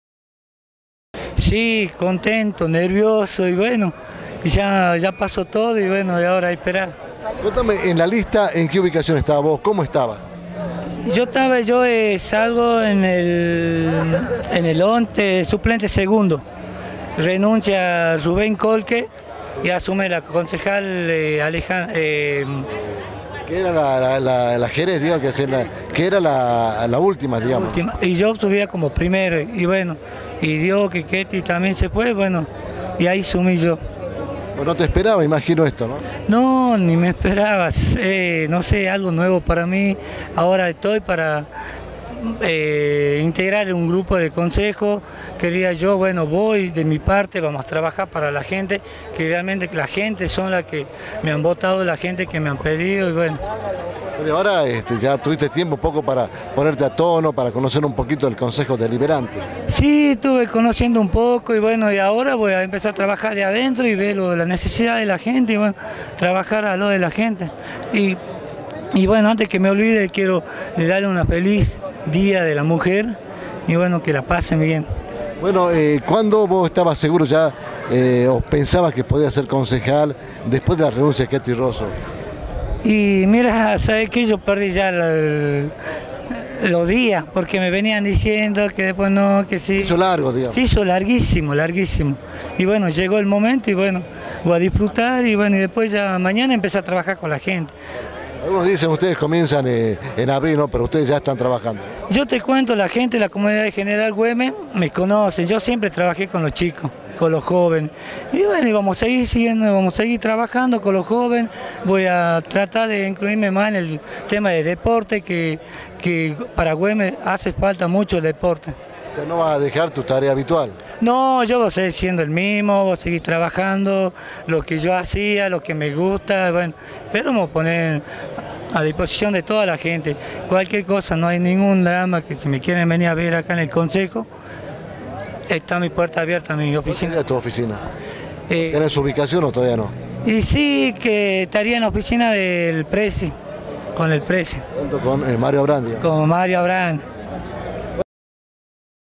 Carlos Rosso ex concejal ya fue reemplazado
Concejal Miguel Maldonado - Asumió como Concejal En Reemplazo de Carlos Rosso – Reemplazara a Carlos Rosso en El Concejo Deliberante